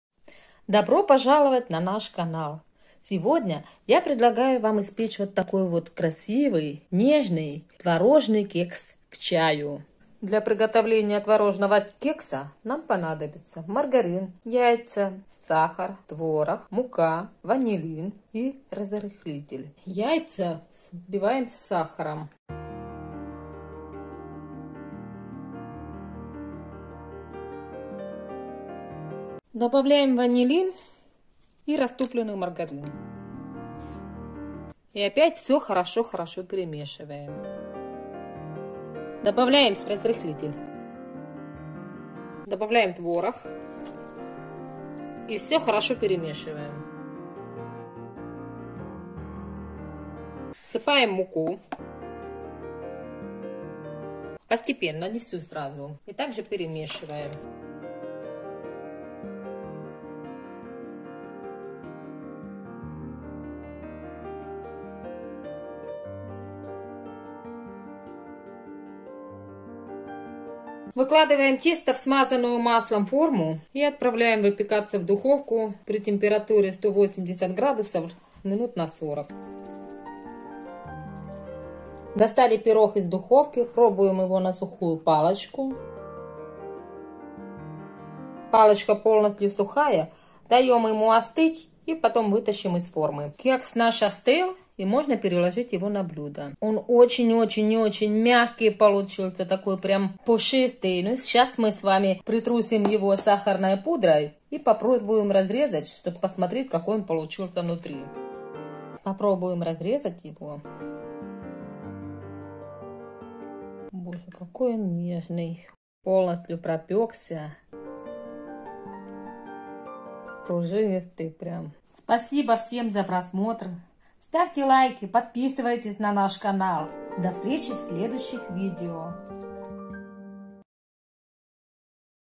кекс творожный
Нежнейший творожный кекс! Просто тает во рту!!
Ингредиенты:
творог-250 гр
яйца-5 шт
сахар-1 стакан
ванилин
мука-150 гр
маргарин-100 гр
разрыхлитель-1ч.л.(или 1 ч.л.соды погасить уксусом)